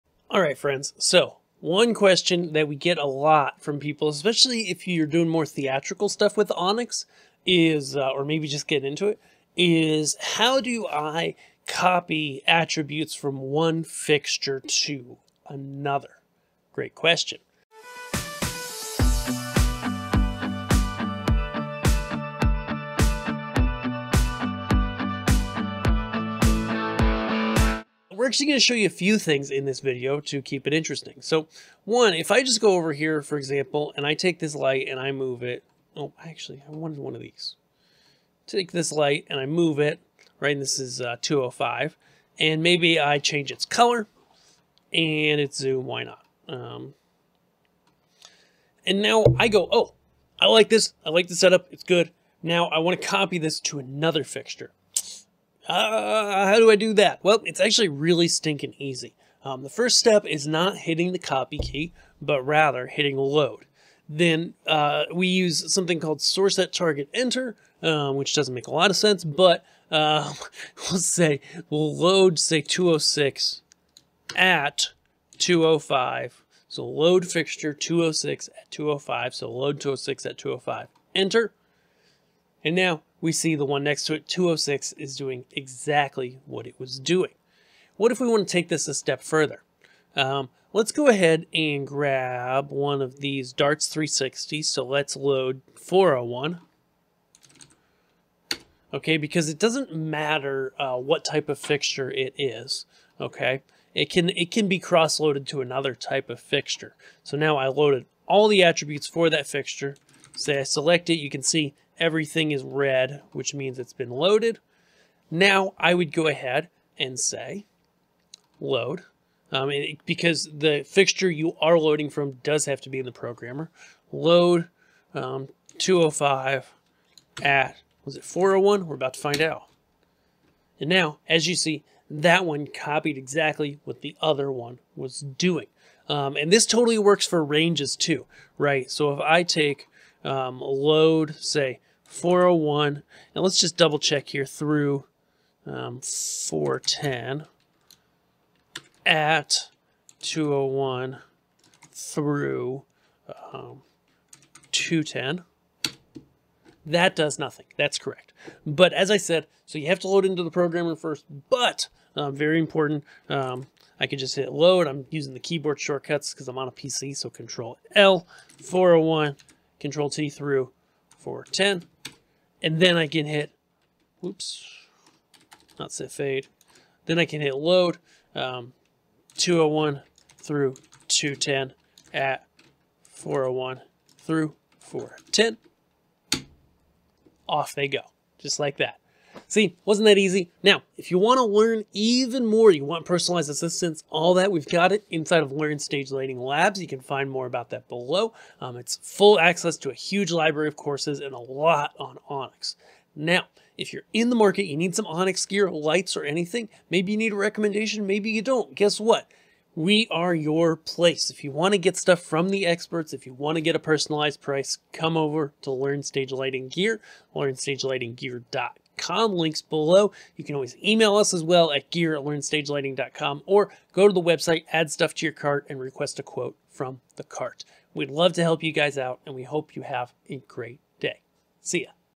How to Copy Attributes from One Fixture to Another
We get asked quite often how to copy attributes from one fixture to another, in this video we show you how to achieve that. What's more, we show you that you can copy ranges, and copy from different types of fixtures to others.

00:00 Introduction
00:32 Copying Attributes
01:30 Copying different type fixtures to each other
02:16 Copying a range of fixtures
03:11 Join the LABS Today for more personalized training!
03:32 Closing

Backdrop/Ambient lighting:
Eliminator Par Mini RGBW

-David Monetization ID TGG0OCXYH5ACR9QY